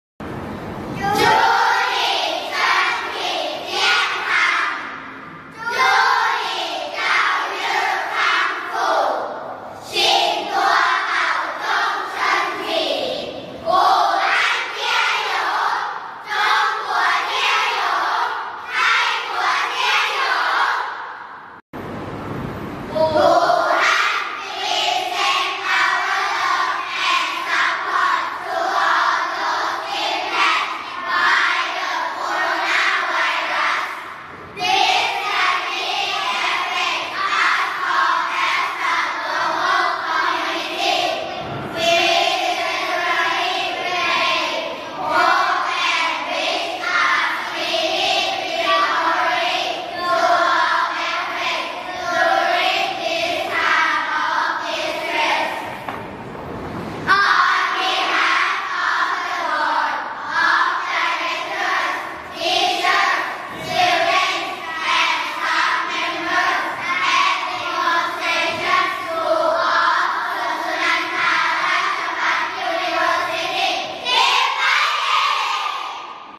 ผู้บริหารโรงเรียนสาธิตสวนสุนันทามีห่วงใยสถานการณ์แพร่ระบาดไวรัสโคโรนา
ผู้บริหาร อาจารย์ เจ้าหน้าที่ และนักเรียนโรงเรียนสาธิตมหาวิทยาลัยราชภัฏสวนสุนันทา มีความห่วงใย​ในสถานการณ์แพร่ระบาดไวรัสโคโรนา ส่งกำลังใจ
祝你身体健康 (zhù nǐ shēntǐ jiànkāng)(จู้ หนี่ เซินถี่ เจี้ยนคัง)ขอให้ร่างกายแข็งแรง
祝你早日康复 (zhù nǐ zǎorì kāngfù)(จู้ หนี่ เจ่า ยื่อ คังฟู่) ขอให้หายป่วยเร็ววัน
请多保重身体！Qǐng duō bǎozhòng shēntǐ ฉิ่ง ตัว เป่าโจ้ง เซินถี่ โปรดรักษาสุขภาพด้วยนะ
武汉加油! อู่ ฮั่น เจีย โหยว  中国加油! จง กั๋ว เจียโหยว  泰国加油! ไท่ กั๋ว เจียโหยว
Wuhan, We send our love and support to all those impacted by the coronavirus. This tragedy affects us all as a global community, we sincerely pray, hope and wish a speedy recovery to all affected during this time of distress.
On behalf of the board of directors, teachers, students, and staff members at Demonstration School of Suan Sunandha Rajabhat University
Keep Fighting!